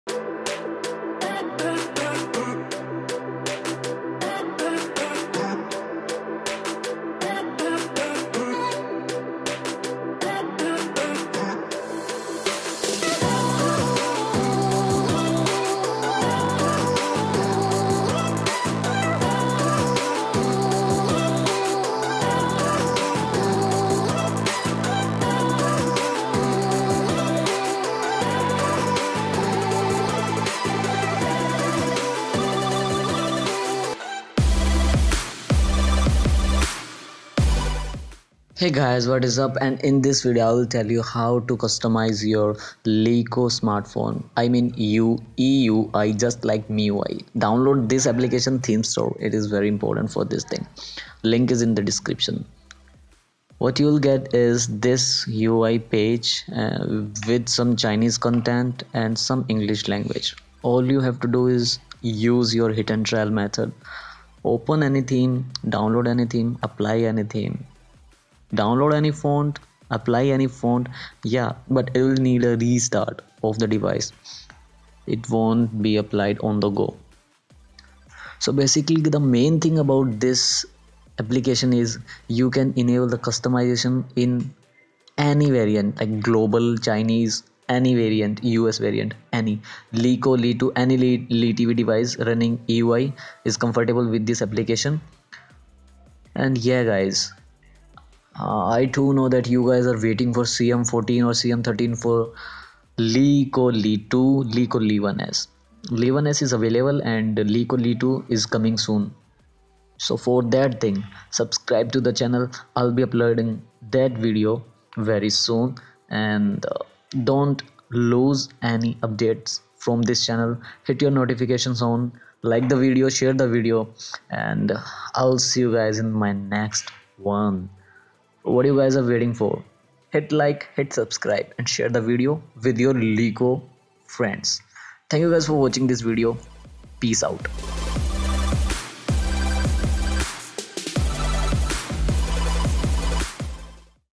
Get iPhone iOS Theme In LeEco LeTV Smartphone! (No Root)
This will enable customization in LeEco devices! Download Theme Store and Use It. Change fonts in EUI.